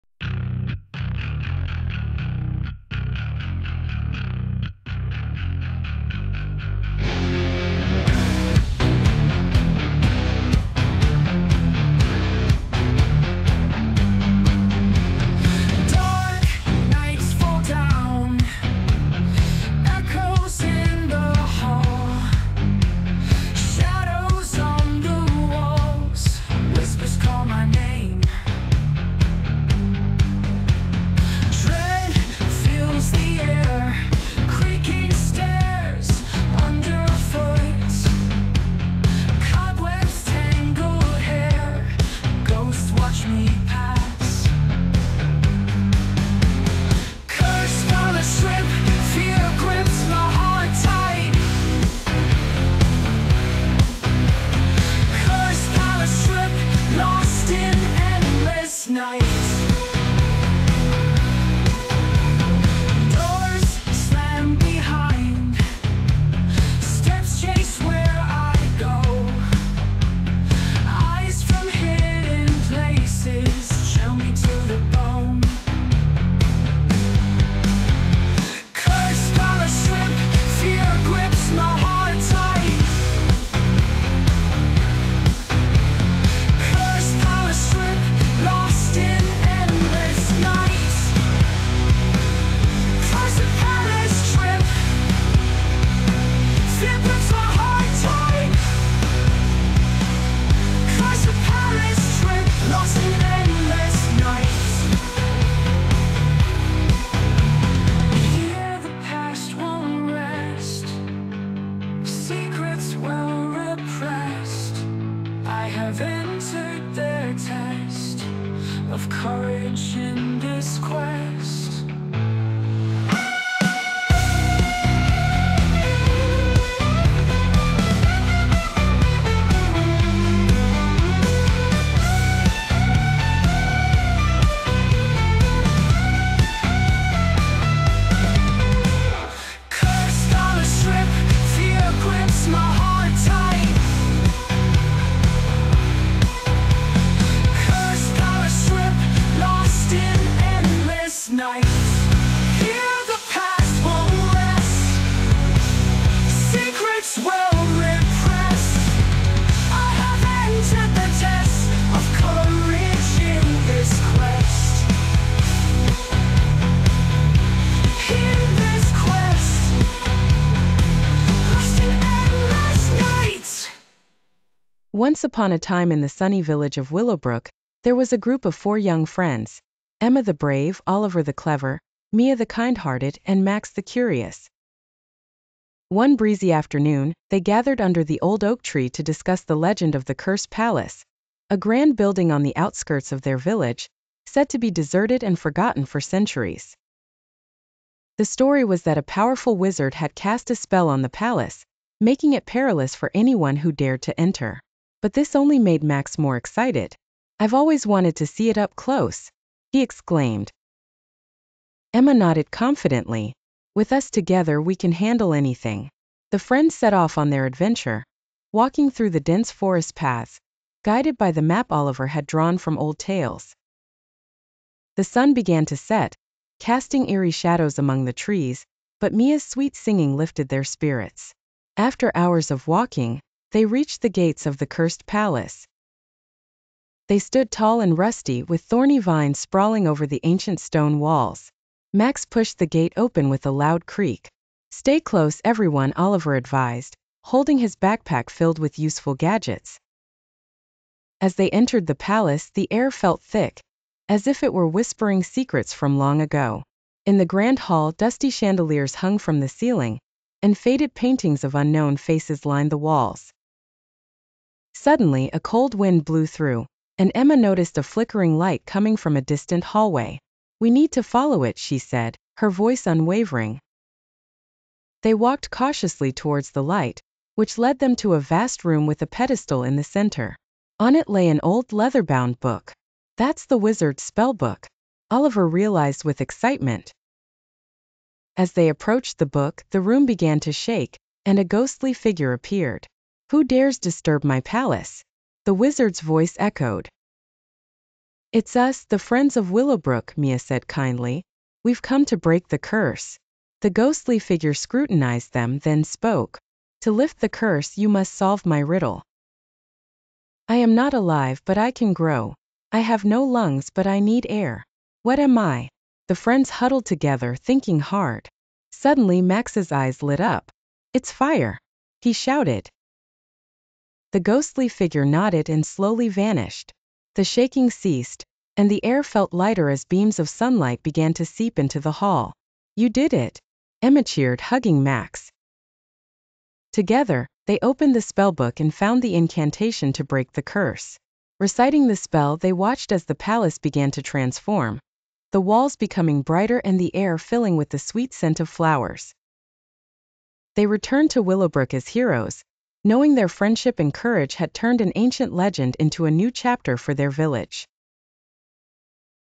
A trip to the cursed palace
Four young friends from Willowbrook embark on a daring quest to break the curse of the abandoned Cursed Palace. With bravery, cleverness, kindness, and curiosity, they solve a riddle, lift the curse, and bring light and hope back to their village